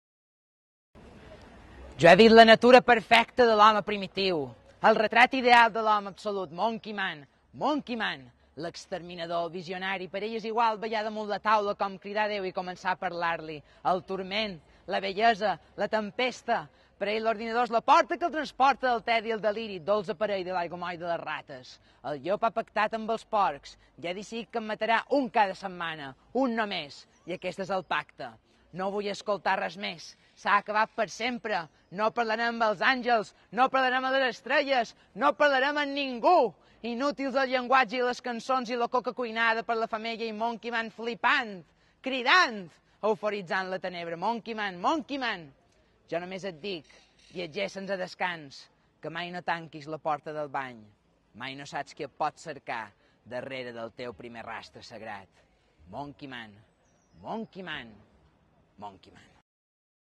Monkeyman a la Plaça del diamant - Punt 1) Ruta poètica per Barcelona 'Unlimited Sobrassada'
Primer esborrany d'edició de video i sincronització d'audio | Gravació de la ruta poètica 'Unlimited Sobrassada' per Barcelona en motiu de la versió per a iPad del llibre de poesia il·lustrada 'Unlimited Sobrassada' de Cristòfol Pons & Jaume C. Pons Alorda